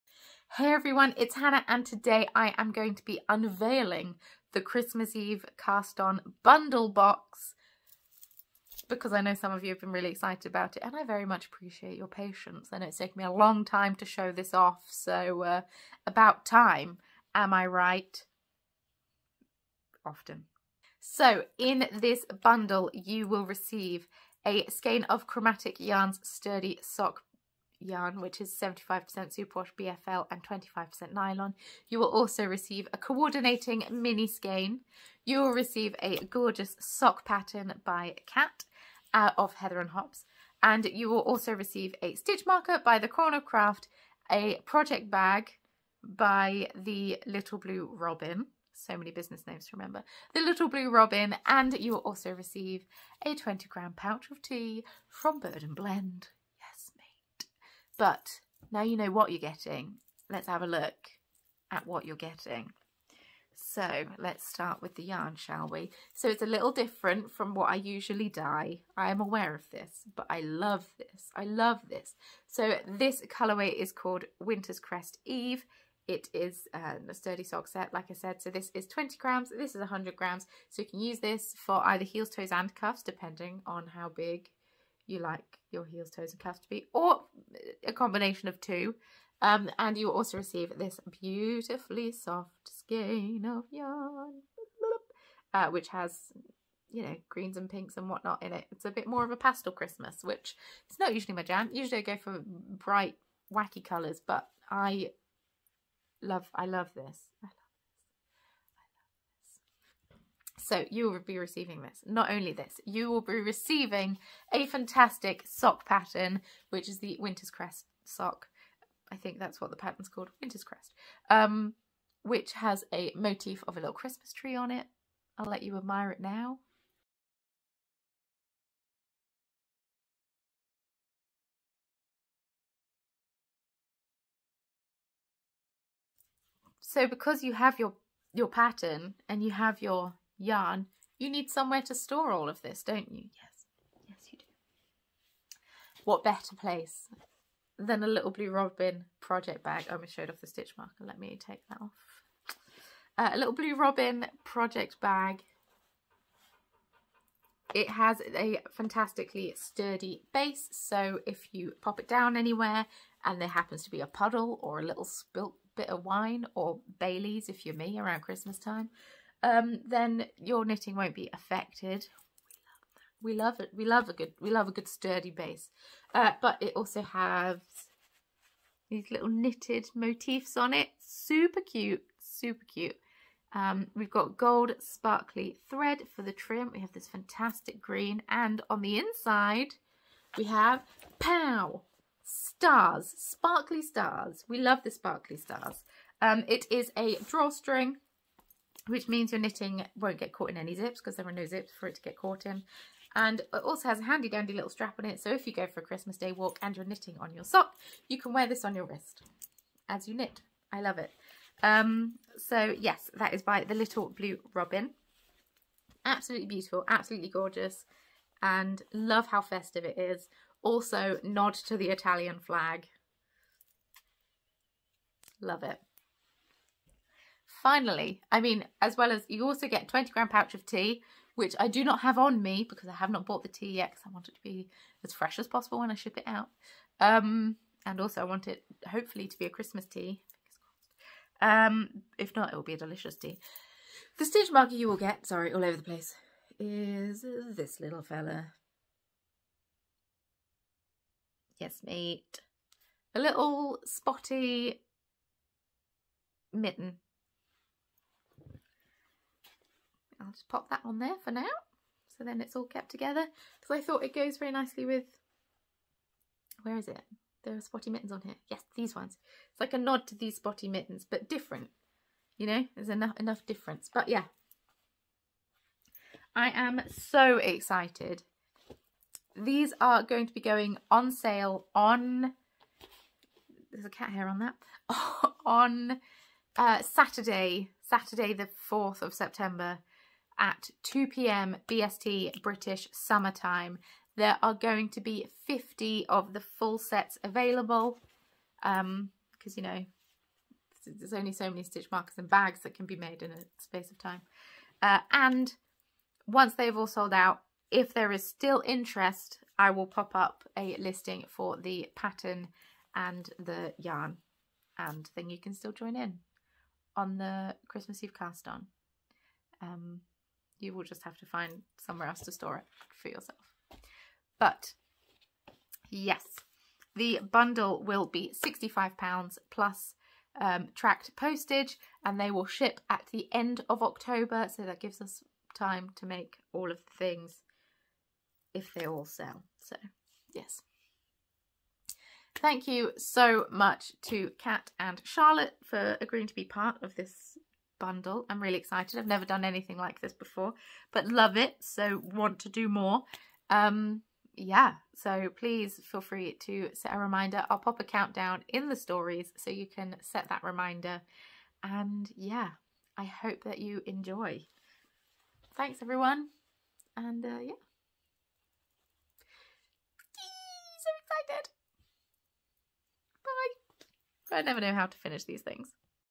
Christmas Eve Bundle REVEAL! ¦ The Corner of Craft | AD
The Christmas Eve Bundle Box Reveal is here!! This is everything you will be receiving if you choose to purchase the Christmas Eve Cast On Bundle.

Bundles will go on sale at 2pm BST (UK time) on Saturday 4th September 2021. They'll be £65 plus tracked postage and will be shipping in the last week of October. I'm so excited to  bring these to you and I hope you are too!

Get your hands on some stitch markers and Chromatic Yarns